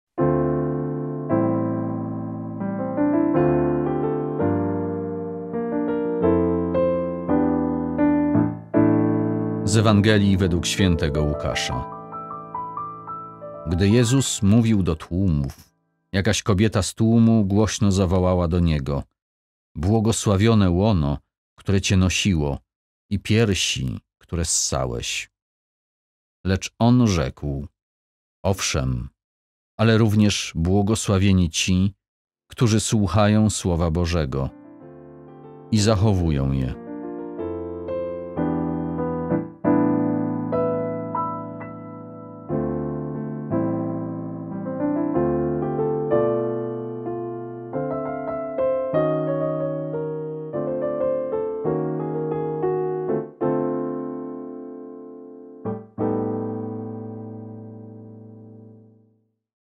Ewangelia | 15 sierpnia 2020 | Msza wigilii
Gdy Jezus mówił do tłumów, jakaś kobieta z tłumu głośno zawołała do Niego: «Błogosławione łono, które Cię nosiło, i piersi, które ssałeś».
Lecz On rzekł: «Owszem, ale również błogosławieni ci, którzy słuchają słowa Bożego i zachowują je».